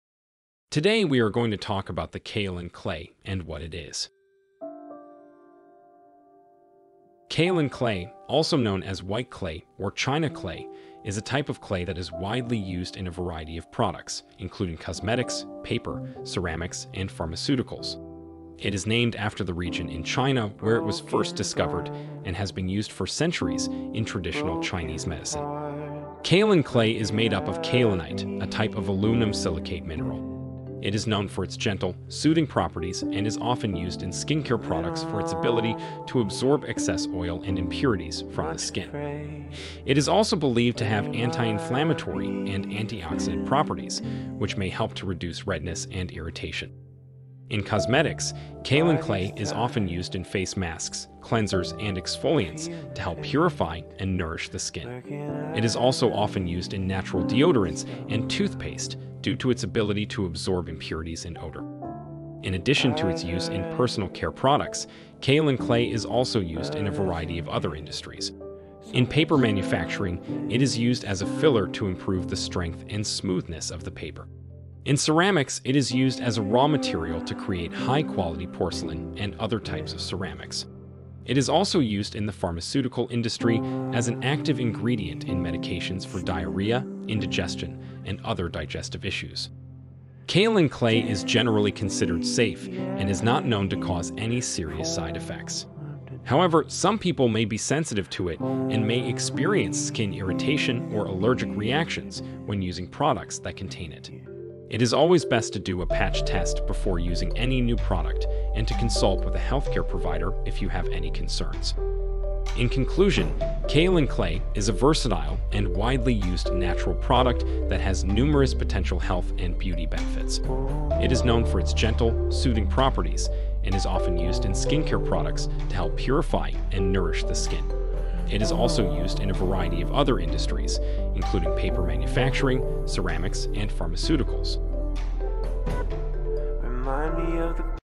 What is kaolin clay?
Kaolin clay, also known as white clay or China clay, is a type of clay that is widely used in a variety of products, including cosmetics, paper, ceramics, and pharmaceuticals. It is named after the region in China where it was first discovered and has been used for centuries in traditional Chinese medicine.